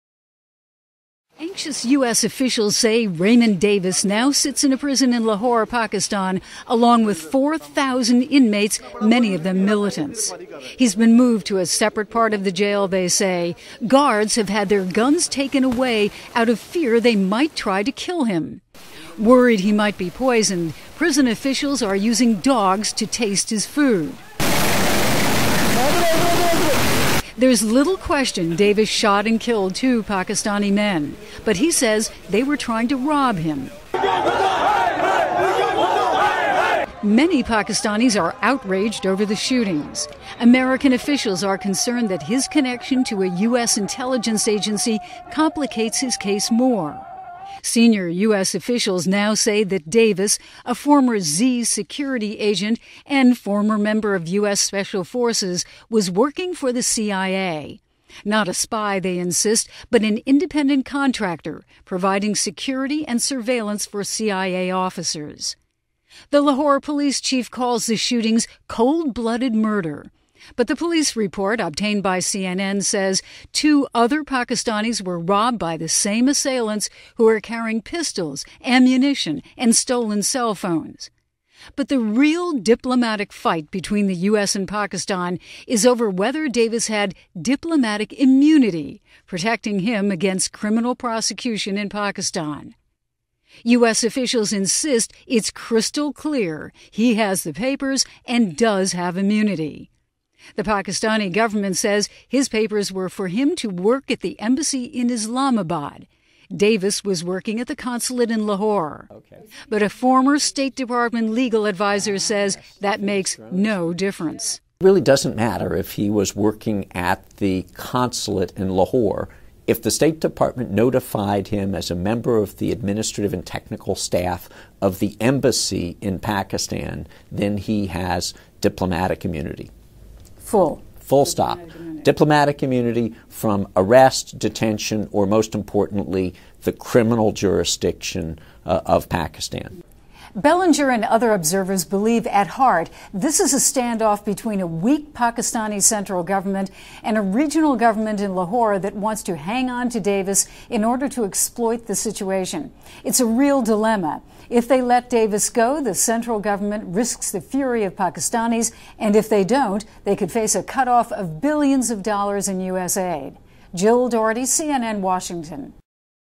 American's CIA ties stirs Pakistanis.
The U.S. says Pakistan must, by law, free an American diplomat accused of murder. CNN's Jill Dougherty reports.